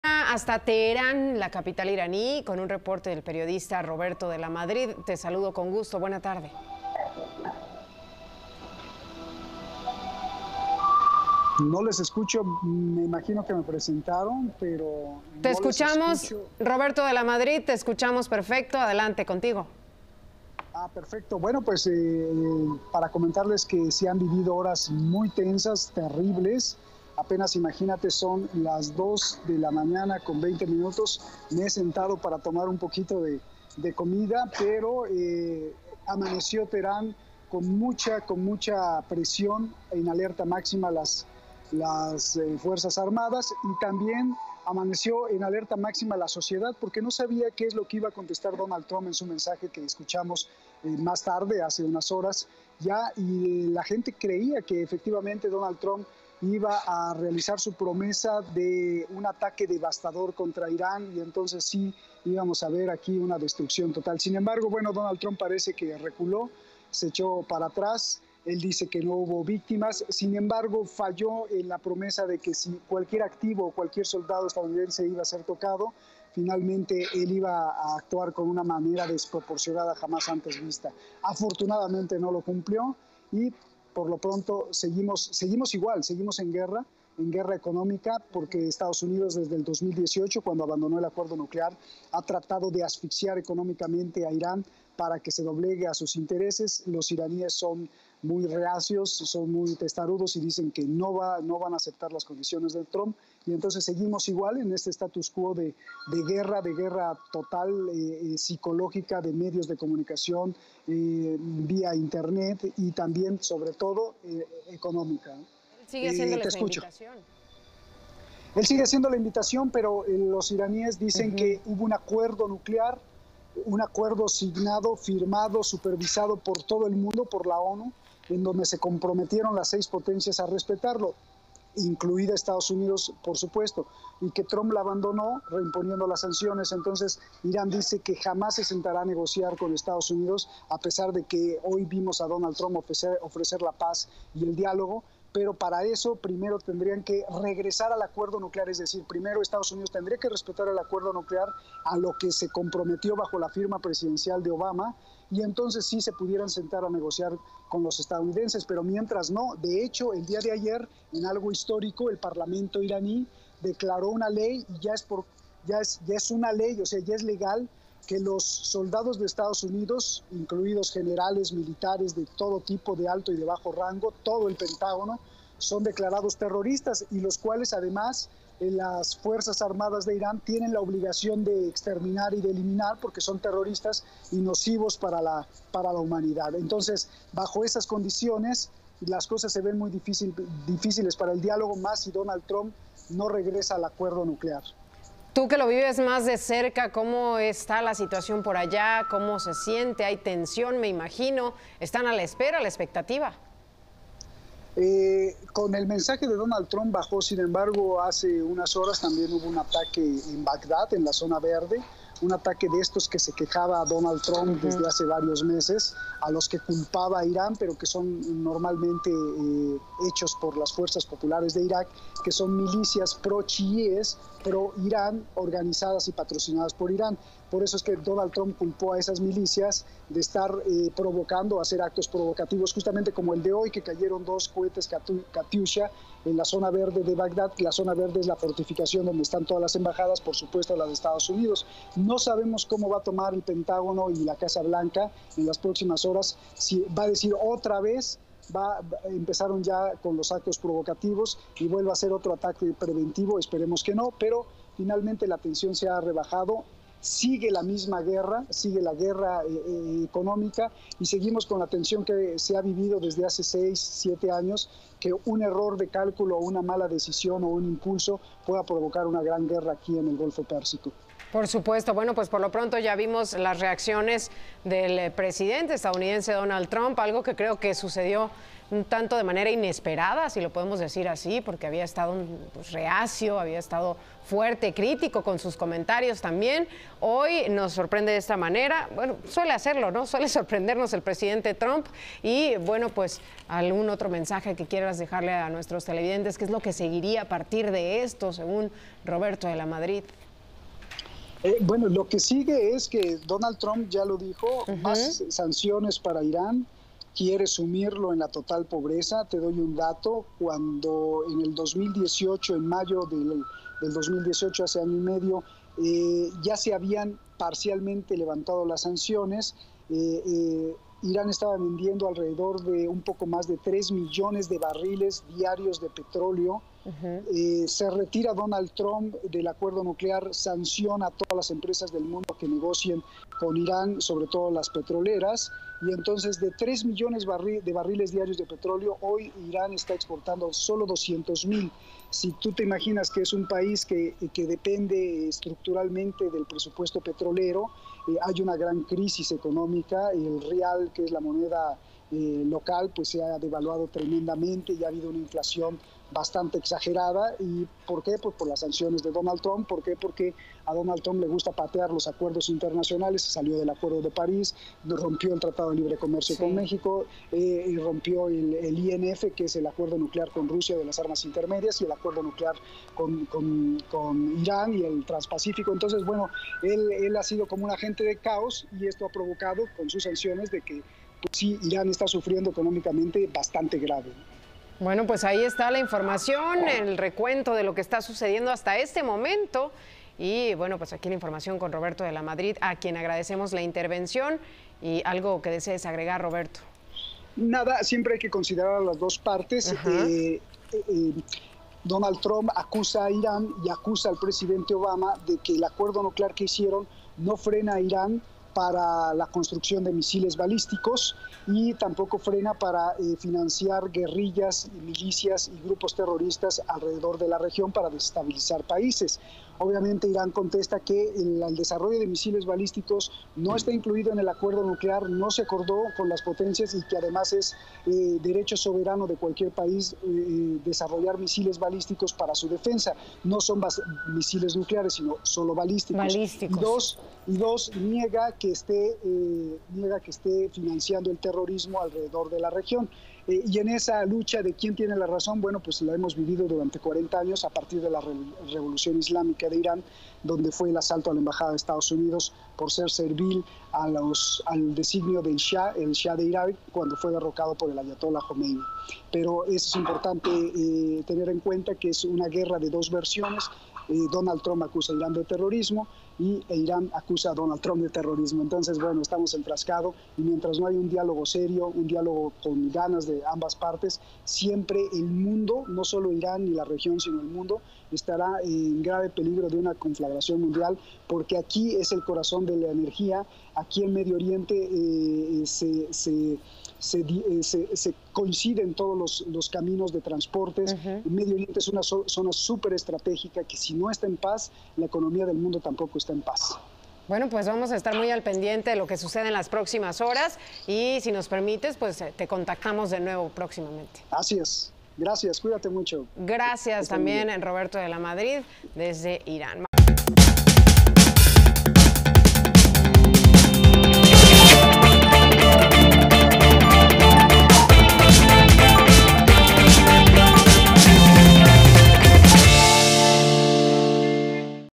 Roberto de la Madrid informa de la situación en Irán
Roberto de la Madrid, periodista, habla en entrevista sobre la situación que se vive en Irán.

8 enero de 2020